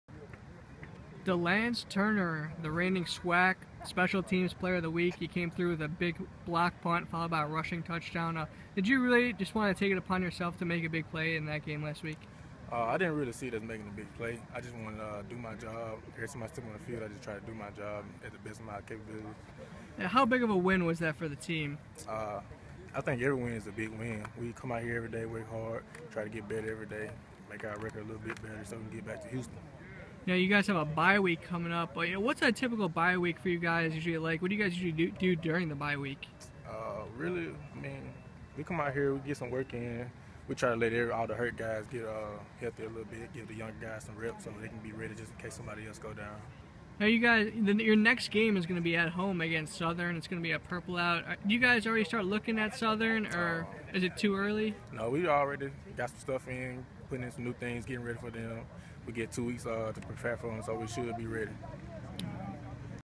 De'Lance Turner - SWAC Special Teams Player of the Week
De'Lance Turner was named SWAC Special Teams Player of the Week on Oct. 17, 2016 after leading the Alcorn State University football program to a 23-20 win over Texas Southern on homecoming.